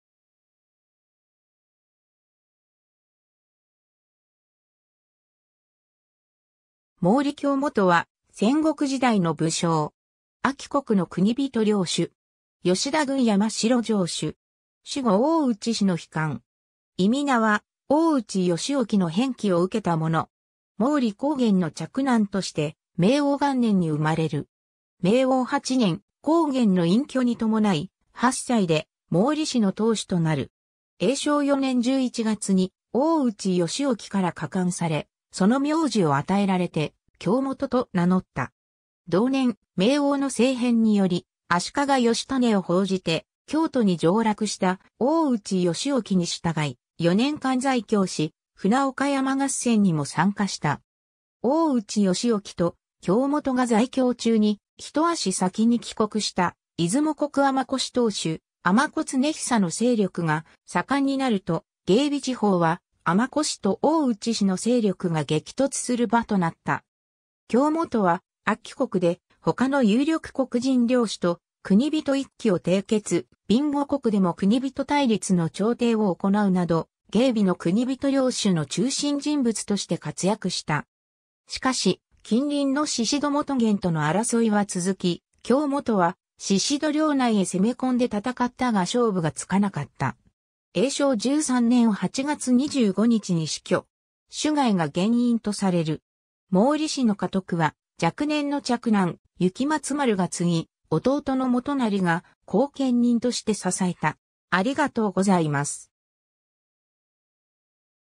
毛利興元
毛利 興元（もうり おきもと）は、戦国時代の武将。
安芸国の国人領主。
吉田郡山城城主。
守護大内氏の被官。
諱は大内義興の偏諱を受けたもの。
 毛利弘元の嫡男として明応元年（1492年）に生まれる。
明応8年（1499年）、弘元の隠居にともない、8歳で毛利氏の当主となる。
永正4年（1507年）11月に大内義興から加冠され､その名字を与えられて興元と名乗った。
 同年、明応の政変により、足利義稙を奉じて京都に上洛した大内義興に従い、4年間在京し、船岡山合戦にも参加した。
大内義興と興元が在京中に、一足先に帰国した出雲国尼子氏当主・尼子経久の勢力が盛んになると、芸備地方は尼子氏と大内氏の勢力が激突する場となった。
興元は、安芸国で他の有力国人領主（高橋久光・平賀弘保・天野興次ら）と国人一揆（安芸国人一揆）を締結、備後国でも国人対立の調停を行うなど、芸備の国人領主の中心人物として活躍した。
しかし近隣の宍戸元源との争いは続き、興元は宍戸領内へ攻め込んで戦ったが勝負がつかなかった。
 永正13年（1516年）8月25日に死去。
酒害が原因とされる。
 毛利氏の家督は若年の嫡男・幸松丸が継ぎ、弟の元就が後見人として支えた。